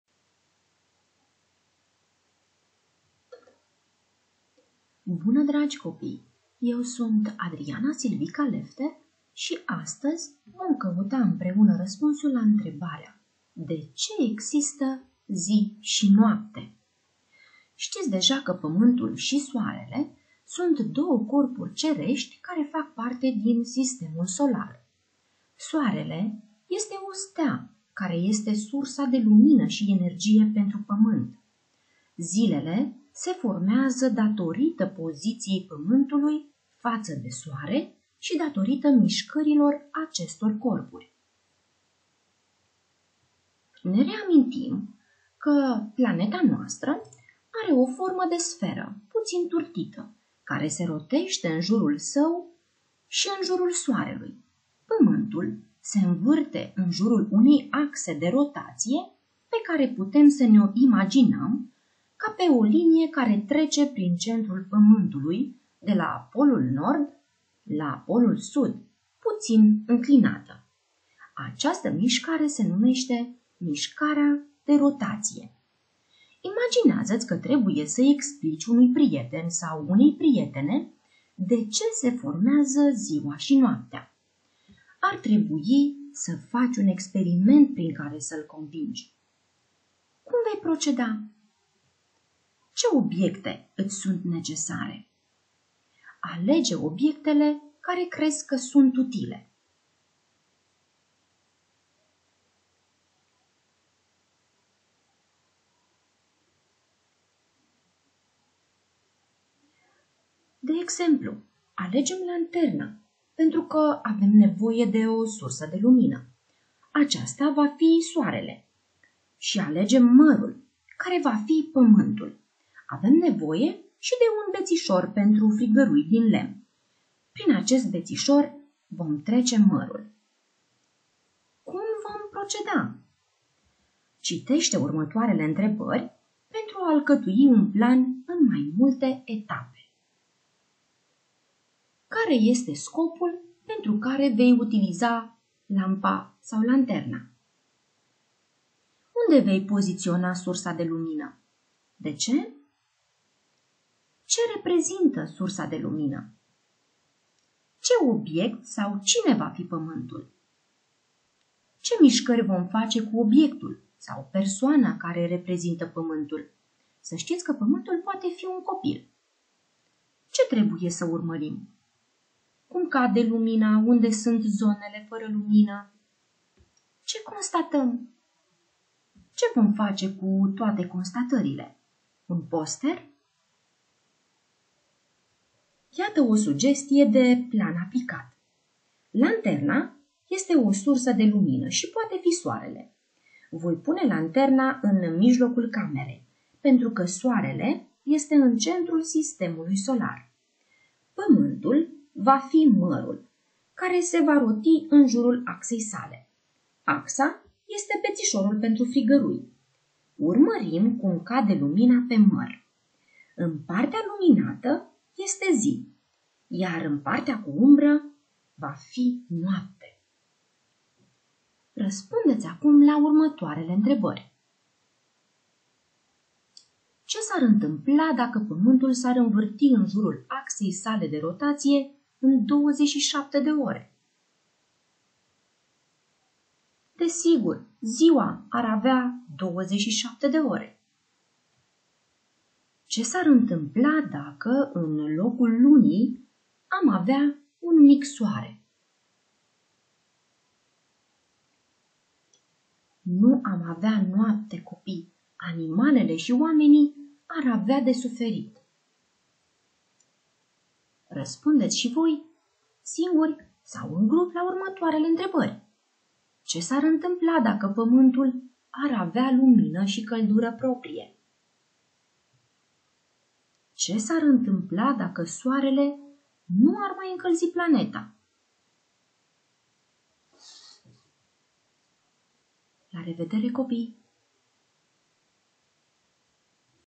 De ce există zi și noapte
Resursa educațională propusă este un material video despre modul în care se formează ziua și noaptea în urma mișcării soarelui în jurul axei sale.

Autor: Adriana Silvica Lefter